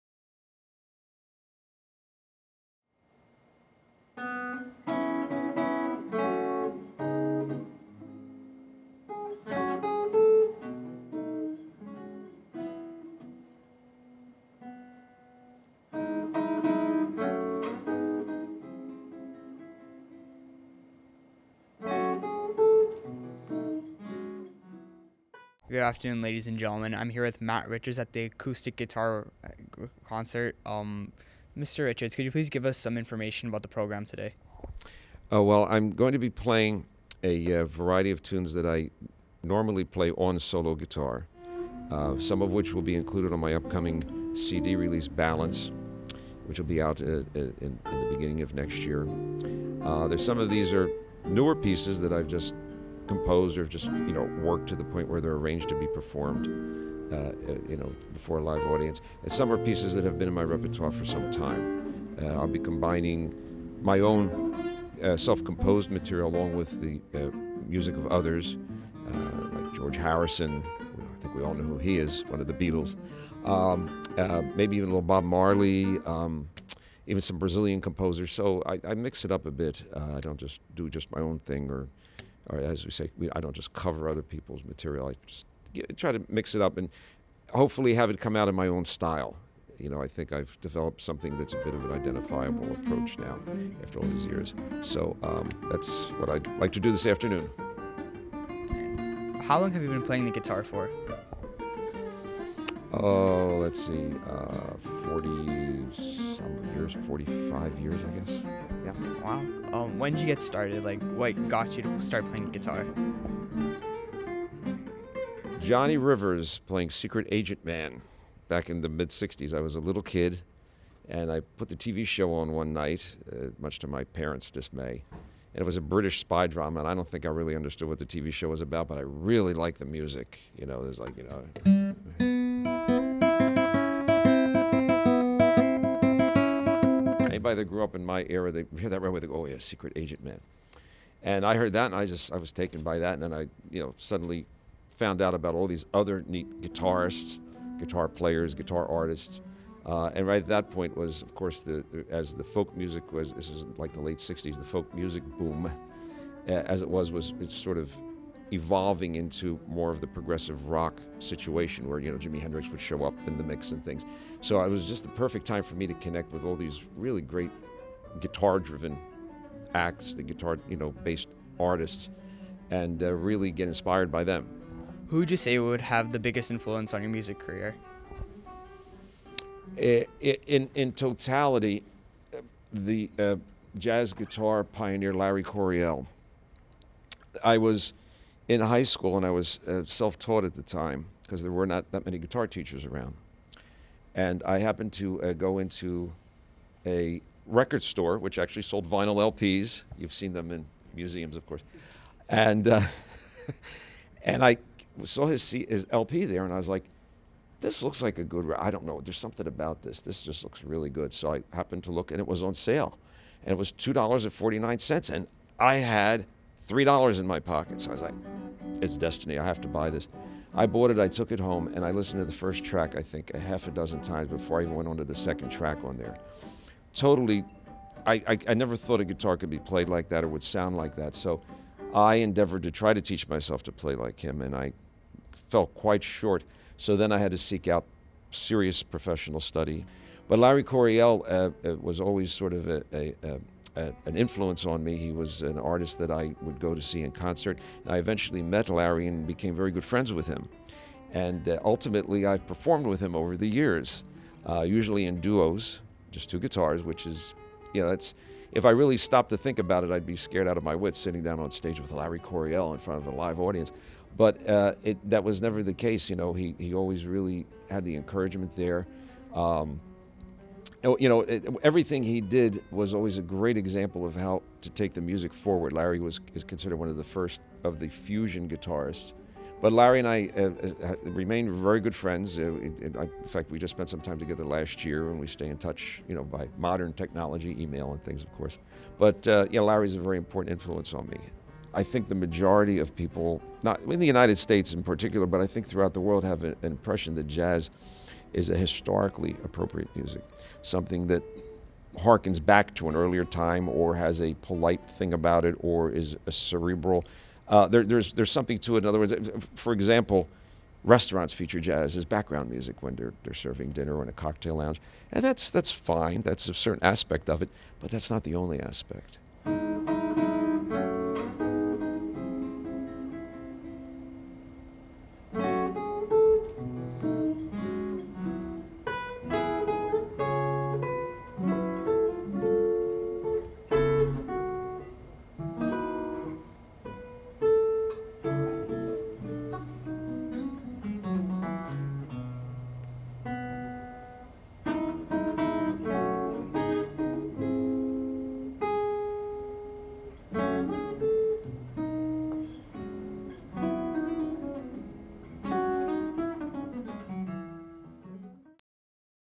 Matt Richards
Matt Richards performs at the East Brunswick Public Library and is interviewed for EBTV.